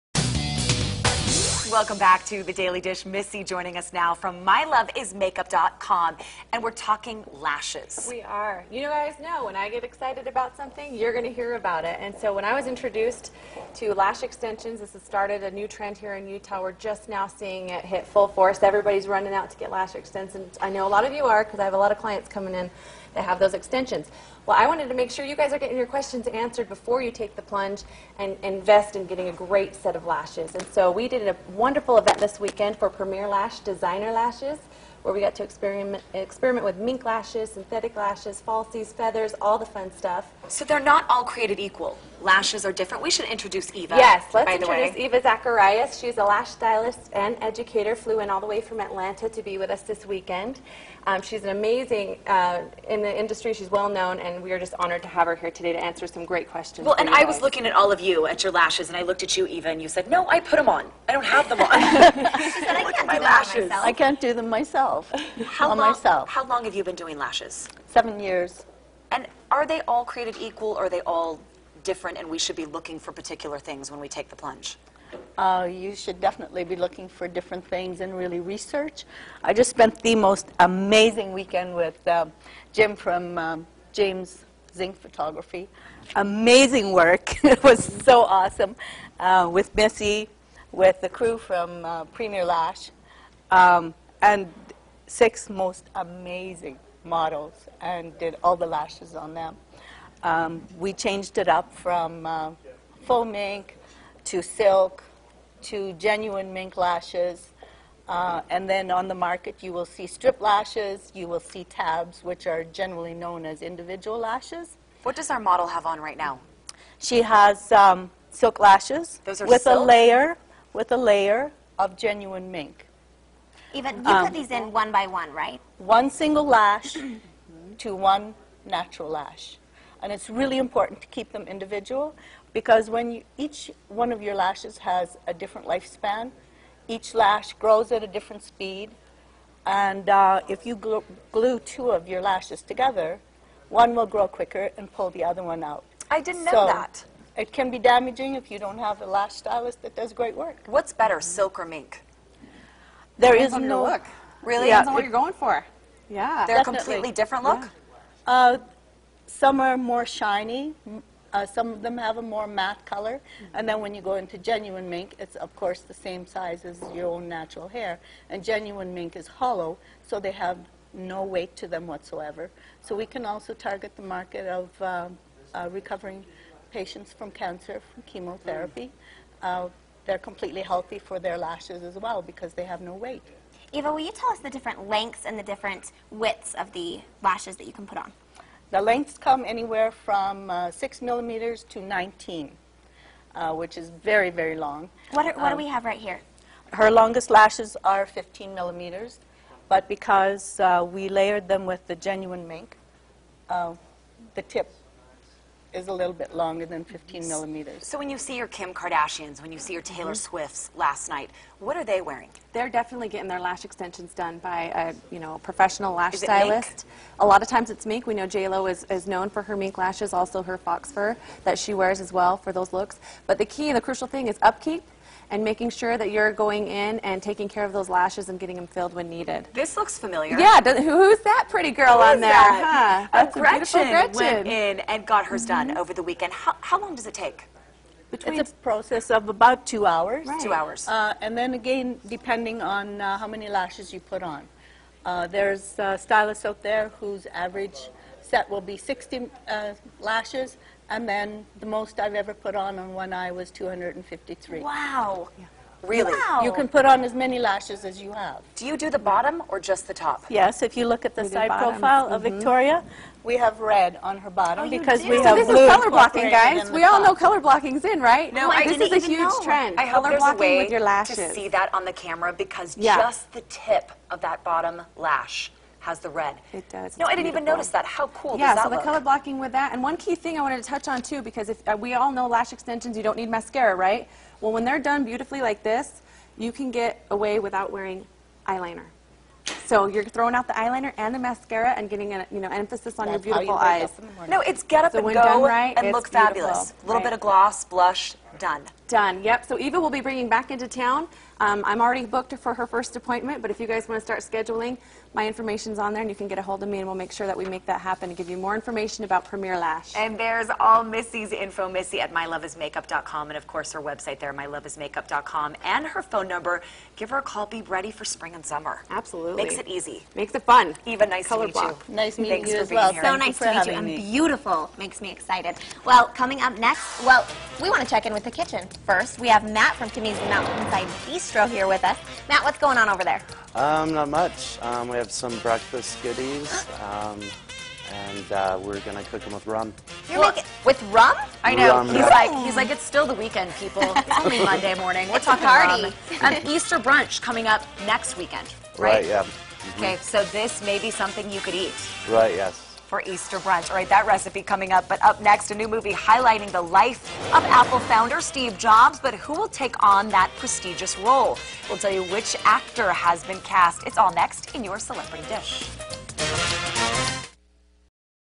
High-End Eyelash Extensions
Eyelash extensions are the hottest trend in make-up right now. Today we are learning about top of the line lashes made from Mink and Fox Fur. Missy Lovett is here to tell you about all the benefits of eyelash extensions and where you can go to get them done.